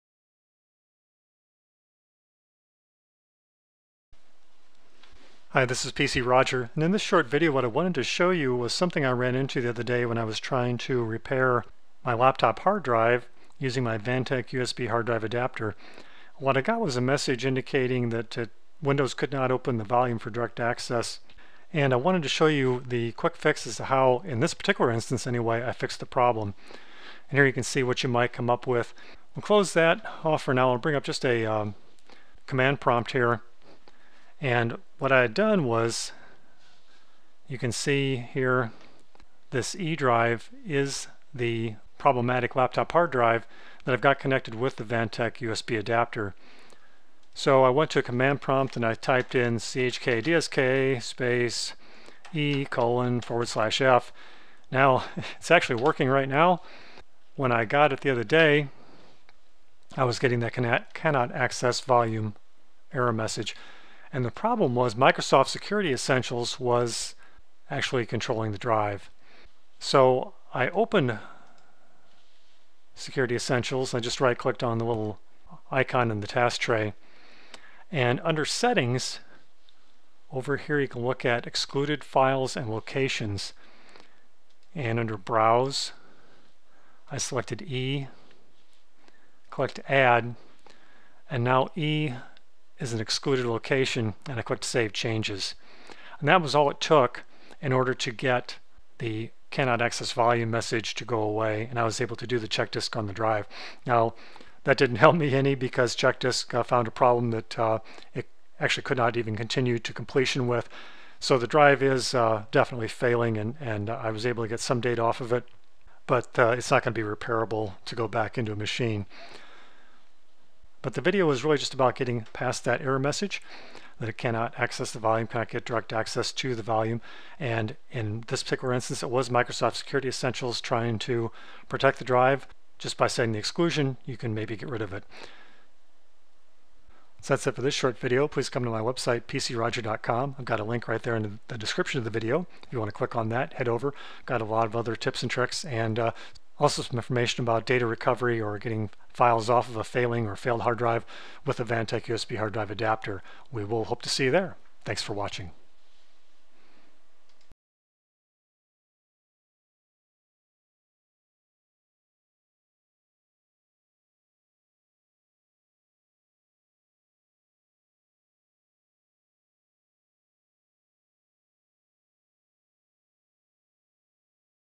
Cannot Open Volume For Direct Access - How To Exclude Drive In MS Security Essentials
- The Vantec USB hard drive adapter is a great tool for easily accessing a hard drive to recover files, perform hard drive

diagnostics or to even repair a hard drive.

Sometimes, though, an antivirus software like Microsoft Security Essentials might prevent full volume access to repair a drive. The error I got was that ChkDsk could not open

volume for direct access.

Learn how to exclude a drive in MS Security Essentials so that diagnostics can get access to the volume and do the repairs.